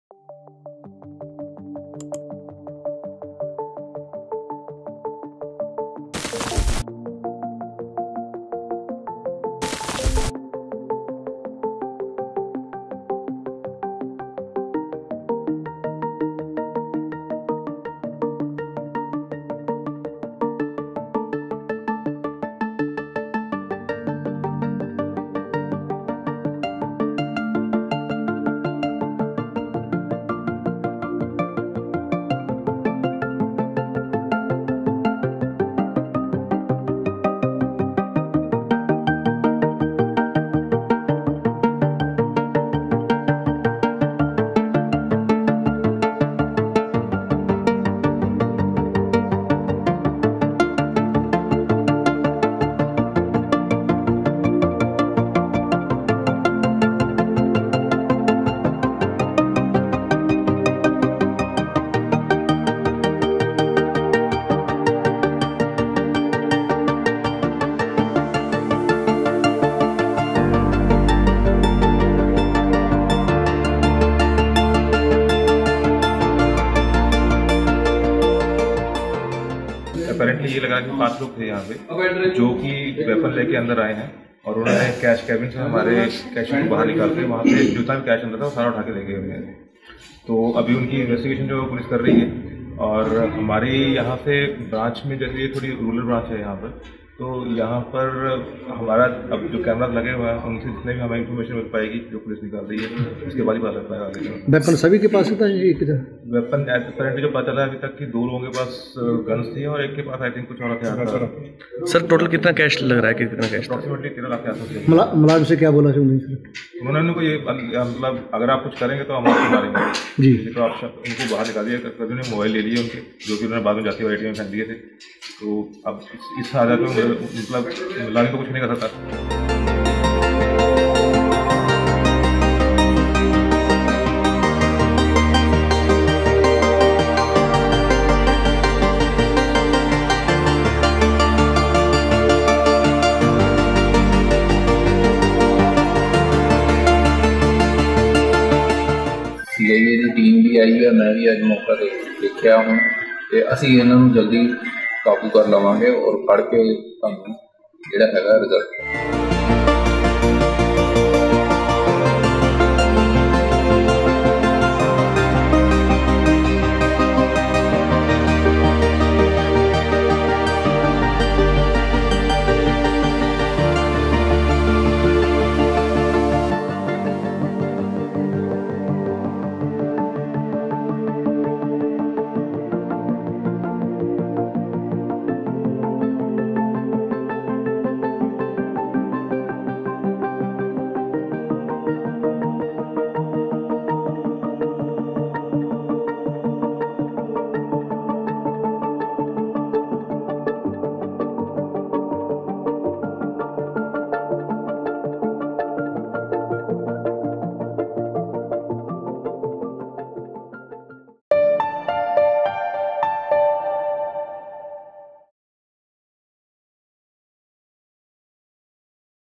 Punjab: Five armed men, four minutes & Rs 13 lakh; Bollywood-style robbery in Ludhiana bank
Five armed robbers looted ₹13 lakh within four minutes from Punjab National Bank's branch in Detwal village of Mullanpur Dakha in Ludhiana on Thursday. The robbers, covering their faces with masks, came on two motorcycles on the pretext of opening a savings account. The incident came as a major embarrassment to Punjab police which had carried out a flag march in the same area hours before the incident. Police are scanning the CCTV footage and have launched an investigation to nab the burglars. Watch this video to know more.